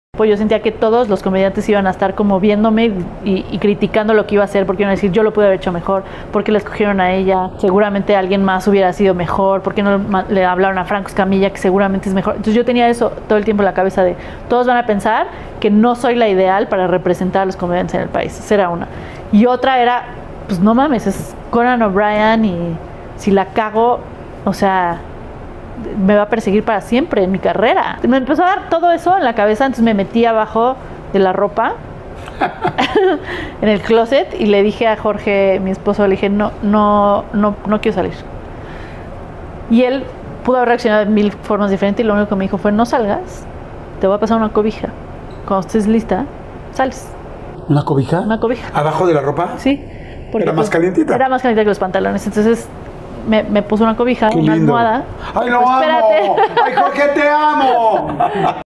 Sofía recibió muchas críticas al abrir el show de ConanOBrien
“Todos van a pensar que no soy la ideal para representar a los comediantes del país” Sofía Niño de Rivera